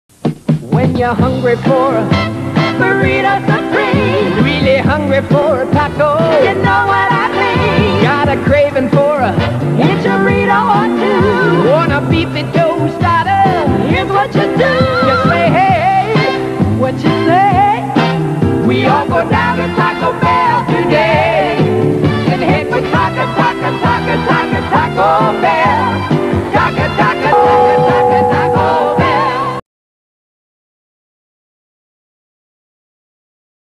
Vintage Taco Bell Commercial (1978)
"Head for taca-taca-taca-Taco Bell!"
Taco Bell commercial featuring the Burrito Supreme, taco, Enchirito, and Beefy Tostada. Spot aired March 1978. for 1970s pop culture fun.*